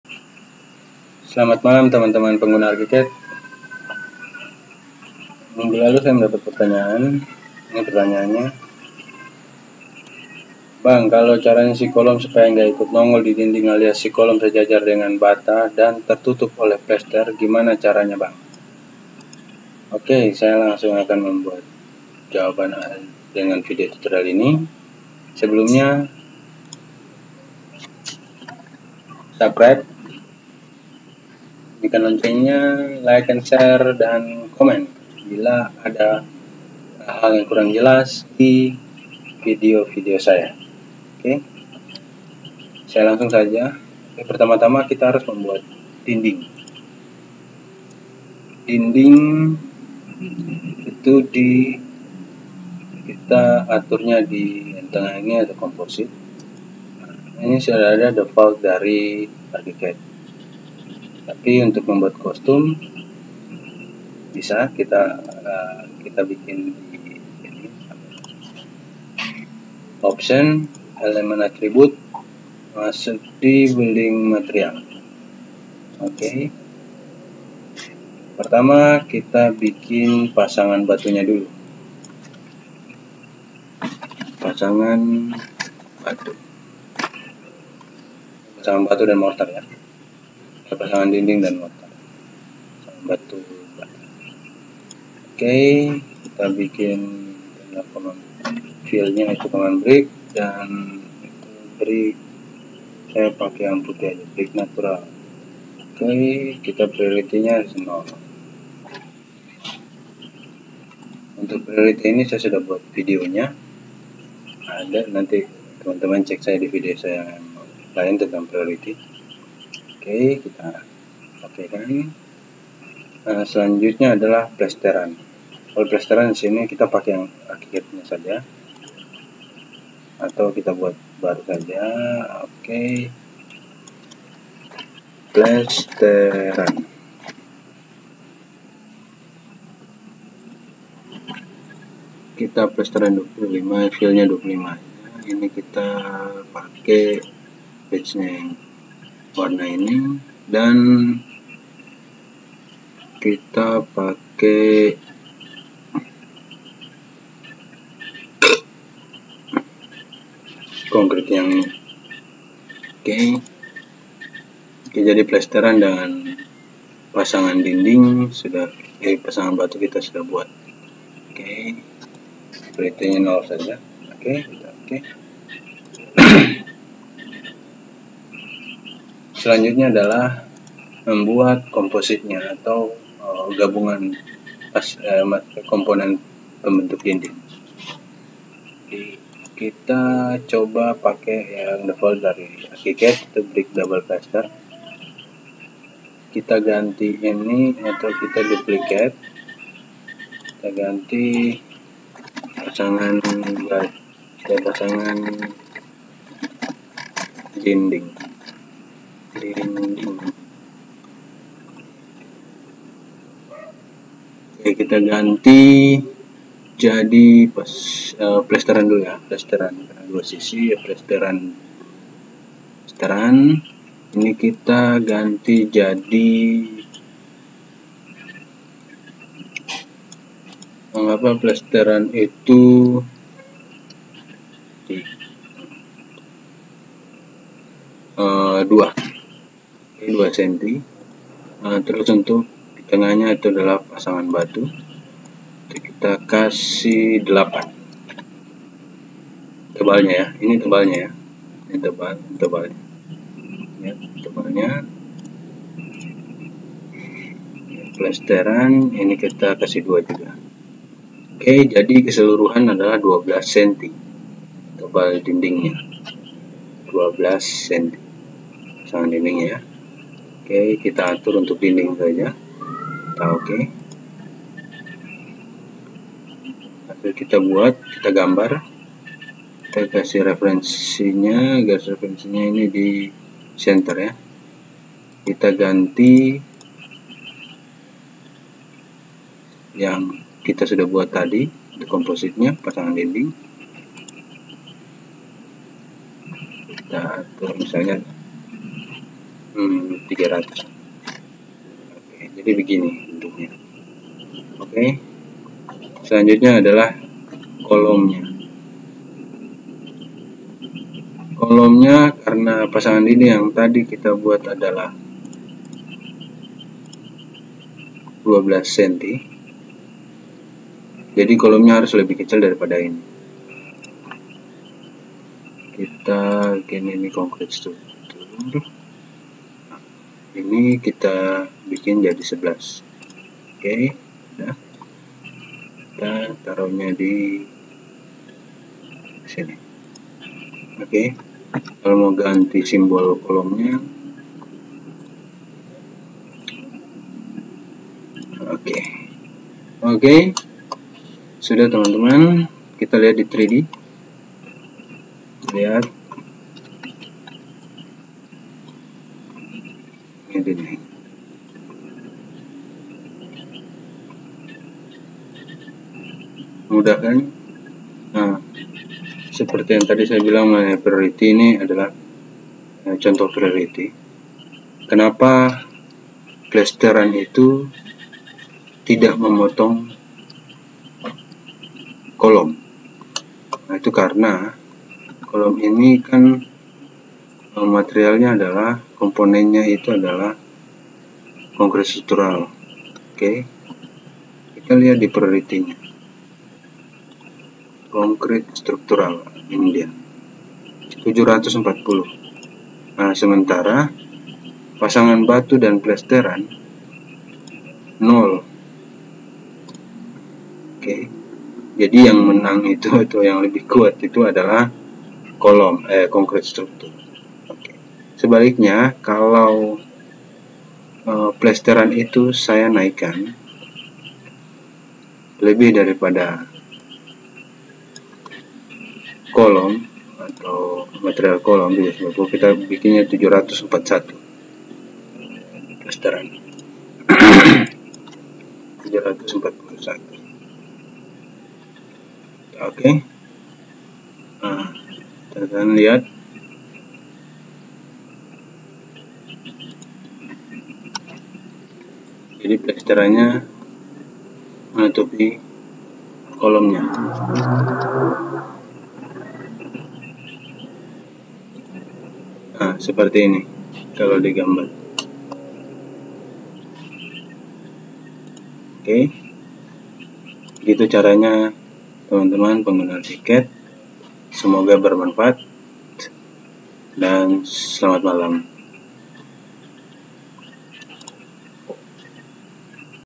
CARA AGAR PLESTERAN DINDING MENUTUPI KOLOM (QnA) ARCHICAD
Agar kolom dapat tertutup dengan plesteran dinding, tebal dinding harus lebih besar dari kolom, untuk merubah tebal dinding dapat diatur di options - element attributes - building materials dan composites seperti yang ada dijelaskan di dalam video ini.